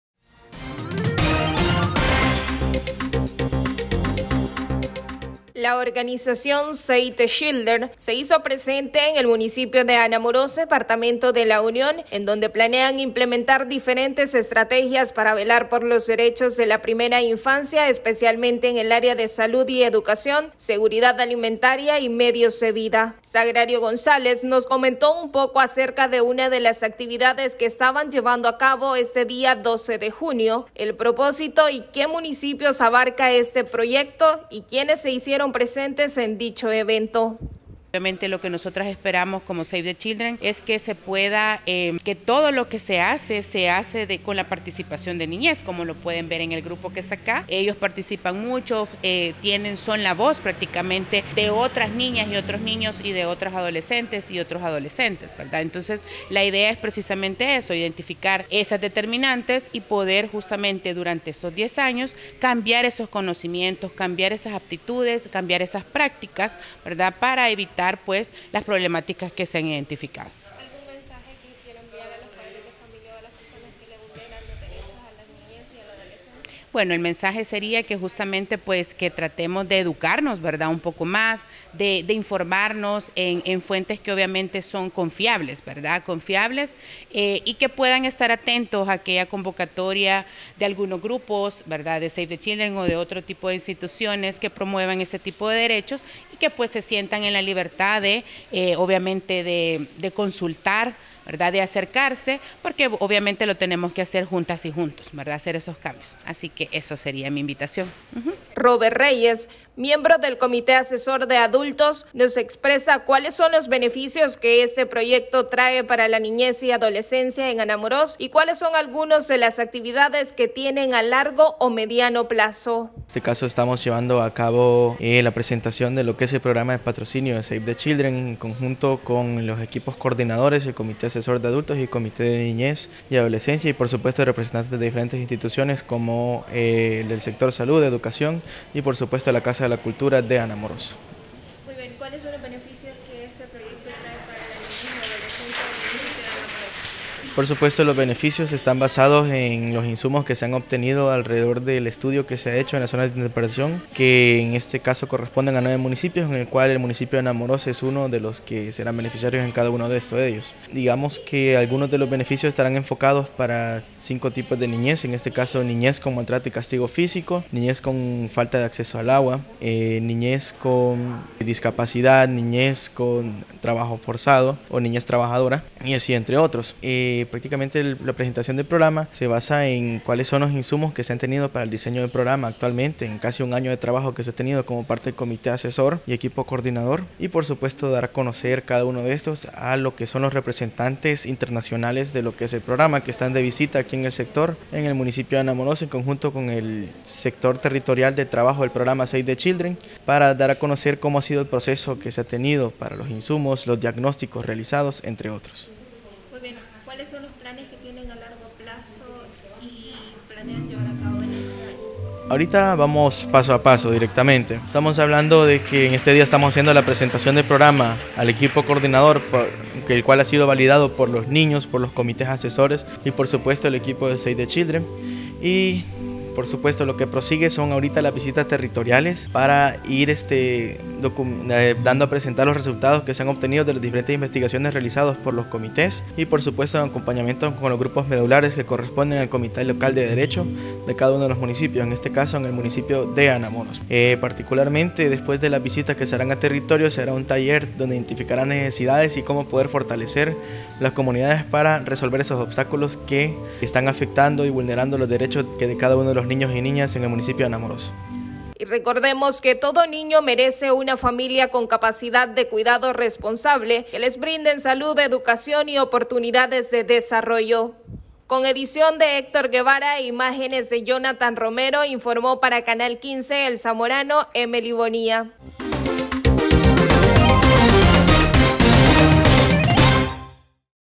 Save the Children se hizo presente en Anamorós, para velar por el derecho de la niñez
Save the Children se hizo presente en el municipio de Anamorós, para velar por el derecho de la niñez